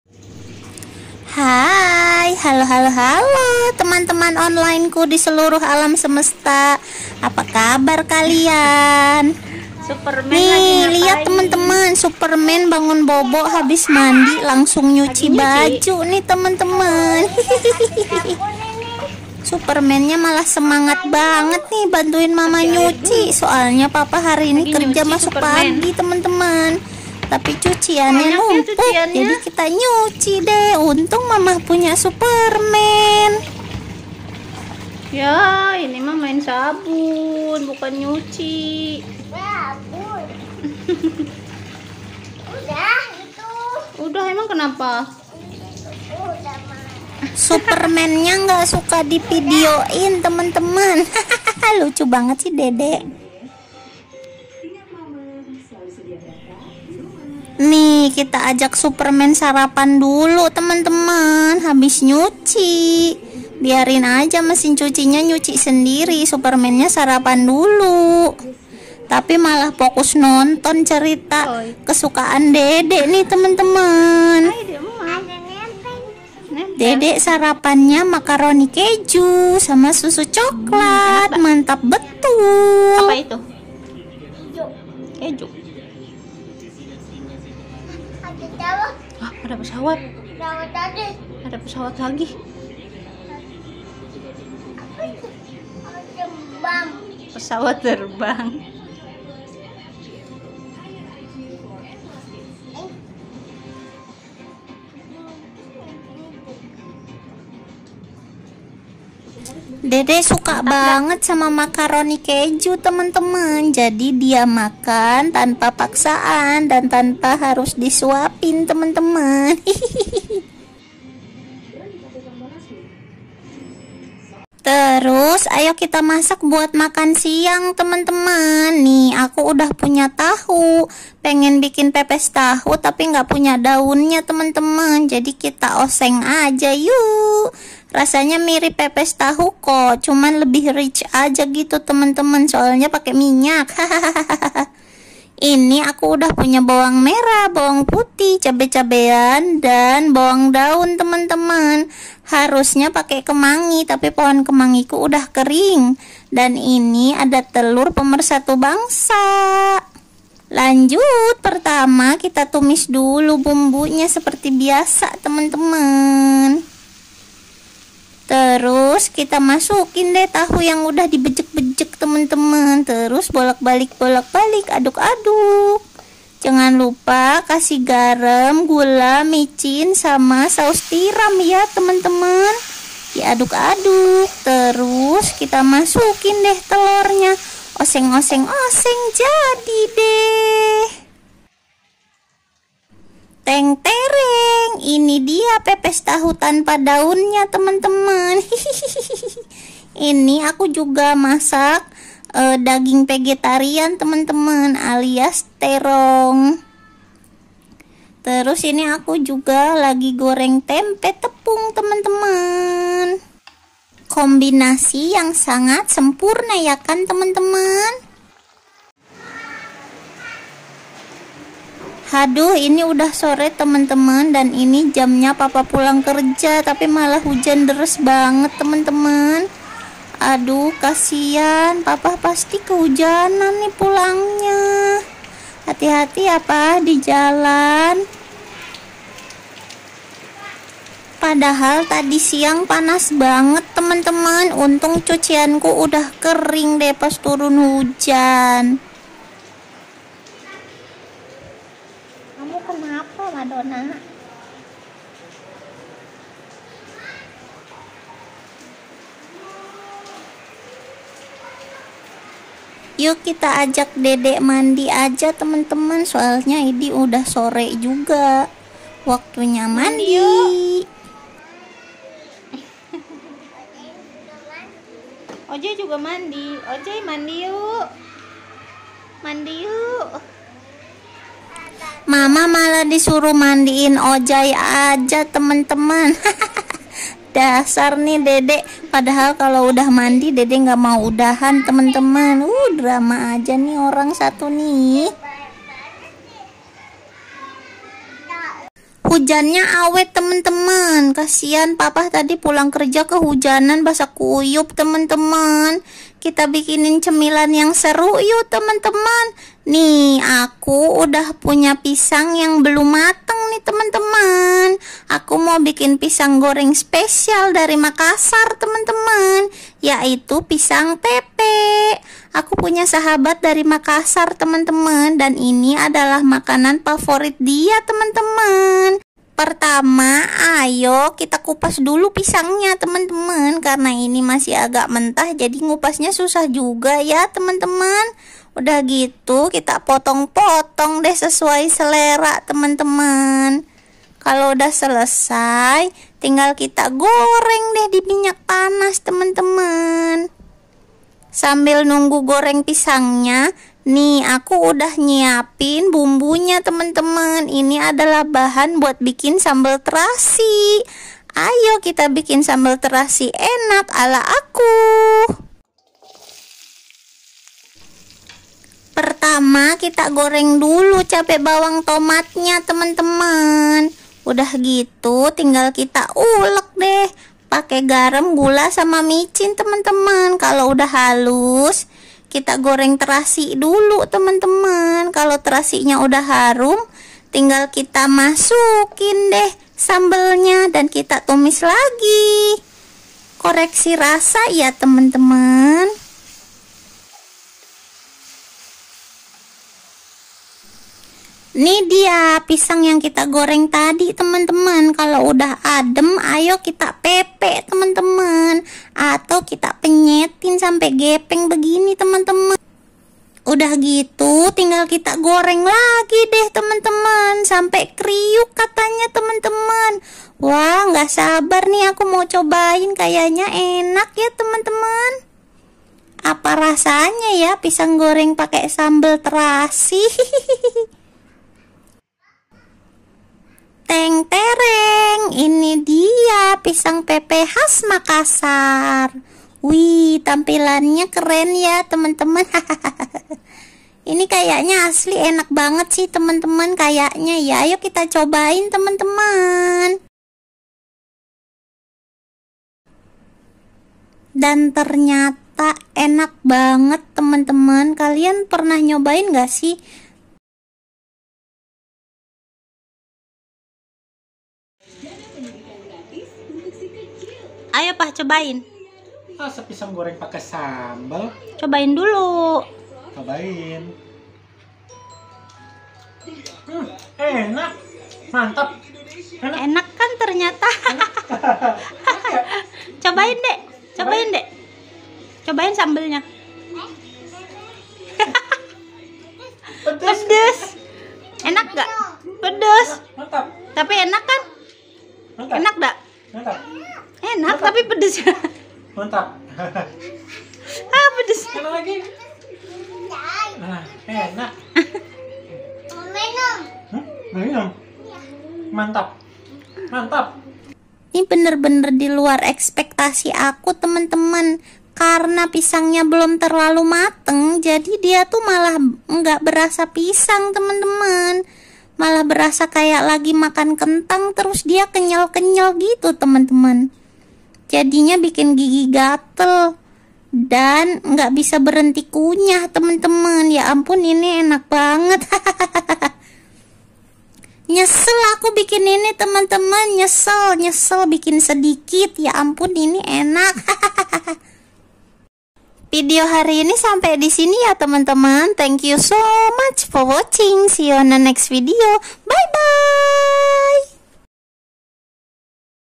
Pisang Goreng Special Pakai Sambel
Ada superman kecil bantu nyuci dan lanjut buat pisang peppe spesial yang enak dan yummy 🤤
Ikuti terus ya semua videonya 😍